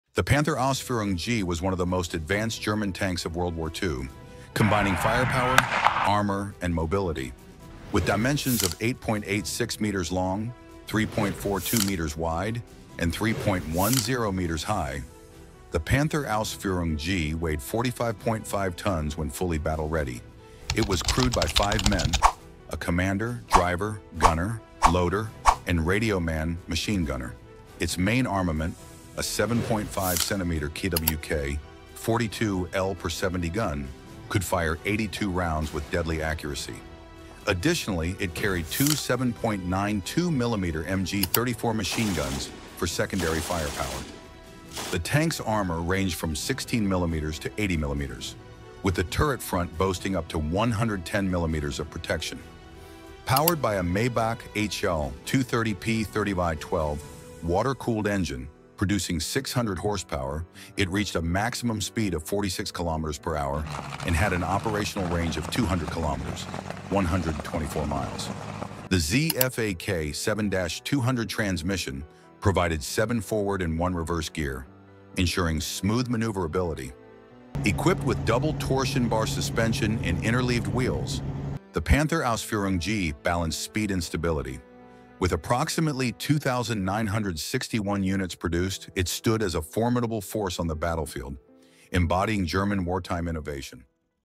Panther Ausf. G – Advanced German Engineering
The Panther Ausf. G was one of the most advanced German tanks of World War II, combining firepower, armor, and mobility.
With dimensions of 8.86 meters long, 3.42 meters wide, and 3.10 meters high, the Panther Ausf. G weighed 45.5 tonnes when fully battle-ready. It was crewed by five men: a commander, driver, gunner, loader, and radioman/machine gunner.
Its main armament, a 7.5 cm Kw.K.42 L/70 gun, could fire 82 rounds with deadly accuracy. Additionally, it carried two 7.92 mm MG 34 machine guns for secondary firepower. The tank’s armor ranged from 16 mm to 80 mm, with the turret front boasting up to 110 mm of protection.
Powered by a Maybach HL 230 P30 V12 water-cooled engine, producing 600 horsepower, it reached a maximum speed of 46 km/h (28.5 mph) and had an operational range of 200 km (124 miles). The ZF AK 7-200 transmission provided seven forward and one reverse gear, ensuring smooth maneuverability.
Equipped with double torsion bar suspension and interleaved wheels, the Panther Ausf. G balanced speed and stability. With approximately 2,961 units produced, it stood as a formidable force on the battlefield, embodying German wartime innovation.